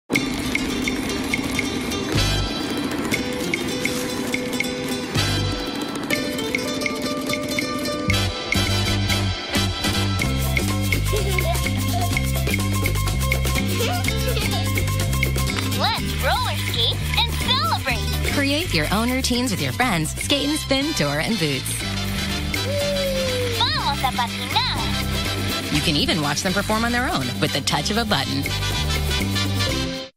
TV Commercial - Fisher Price - Skate & Spin - Dora and Boots - Dora's Great Roller Skater Adventure
Choreograph skate routines for Dora and Boots with your friends or watch them go all on their own.
Super Skaters! These skatemates are inspired by the Nickelodeon television special, Dora's Great Roller Skate Adventure! Watch as Dora & Boots magically skate side-by-side, spin, or skate in a conga line! Kids can even create their own routines for additional hands-on fun! The "wheely" impressive skating routines are complete with upbeat music and English and Spanish phrases!
Advertiser Fisher Price
Mood Active